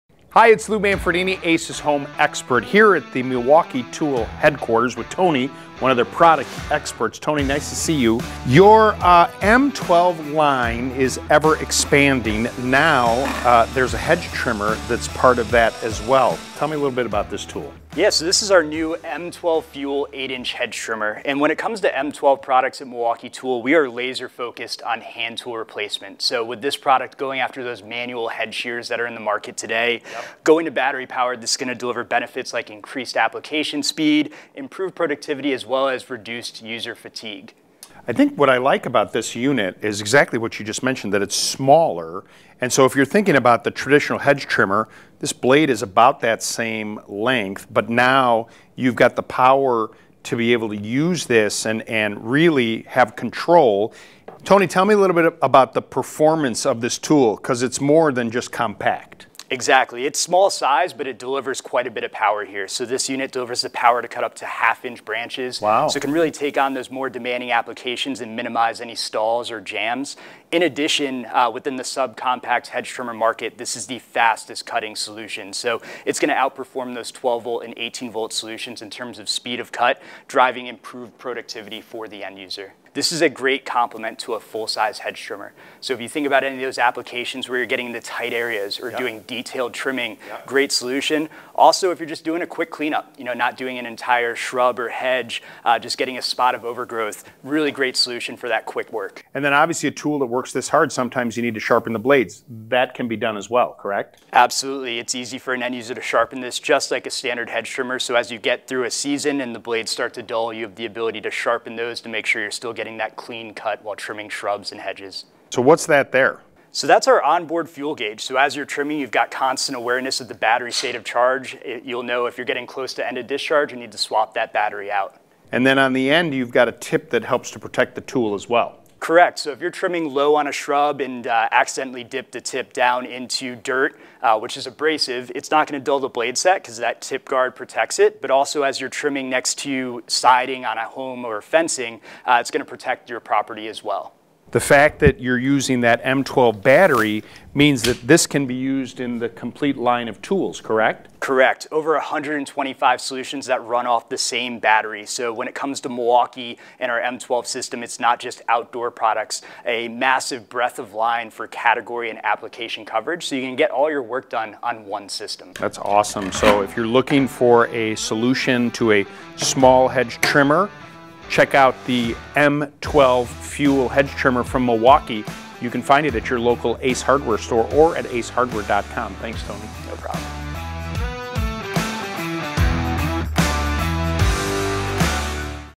M12 FUEL™ 8" Hedge Trimmer Product Overview - Ace Hardware
Watch as Lou Manfredini, Ace's Home Expert, heads to Milwaukee's Headquarters to get all the details about the M12 FUEL™ 8" Hedge Trimmer  2533-21. Learn the features and benefits of this must have outdoor power tool.